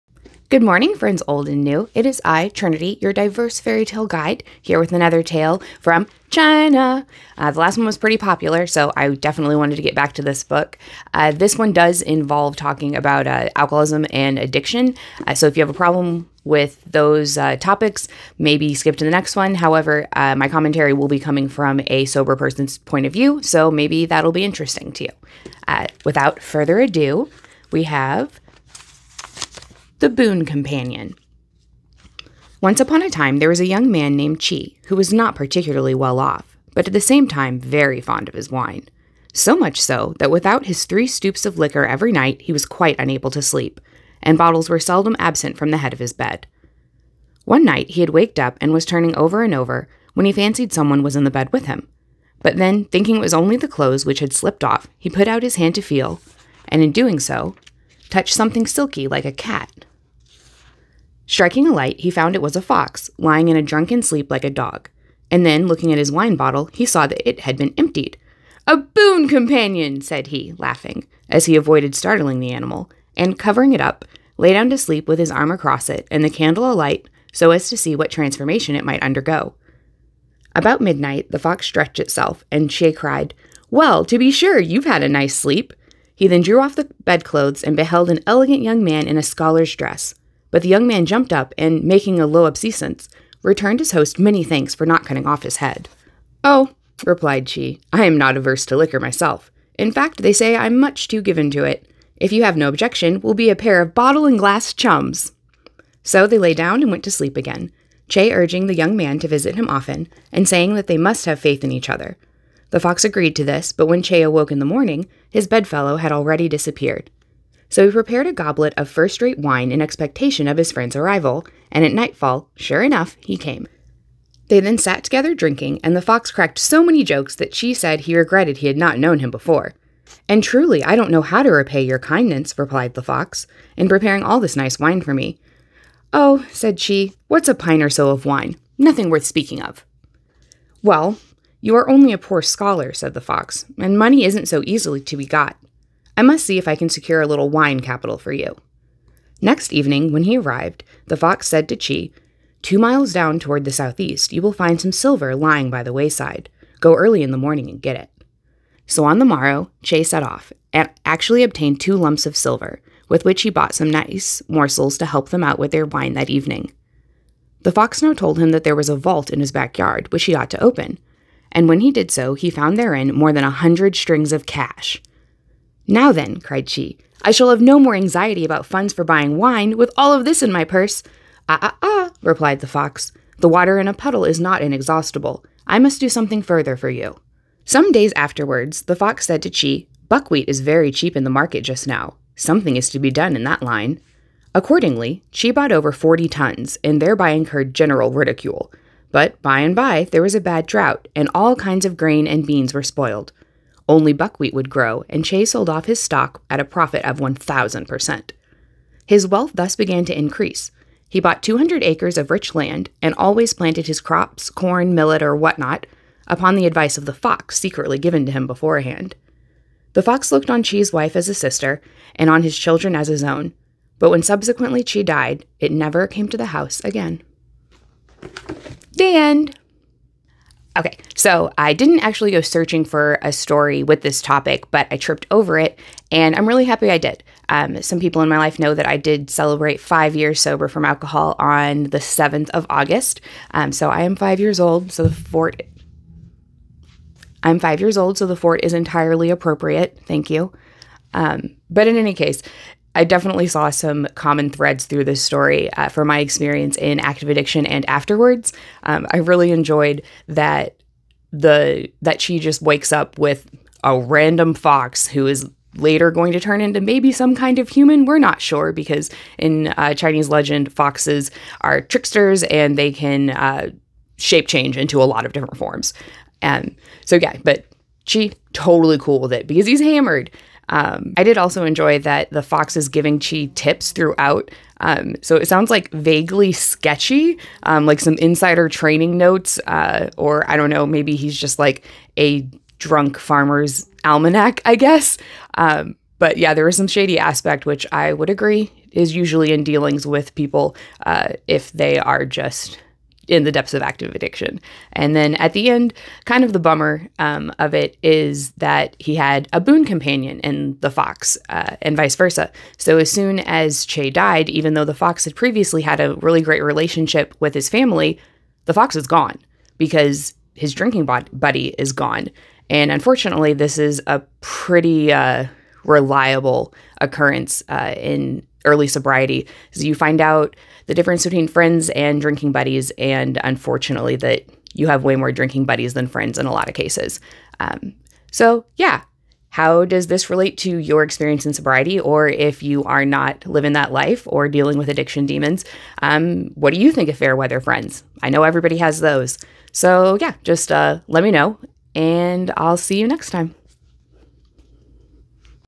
Boon-Time Companion - Chinese Folk Tale - RWM ep. 16
Let's talk trickster foxes and sobriety!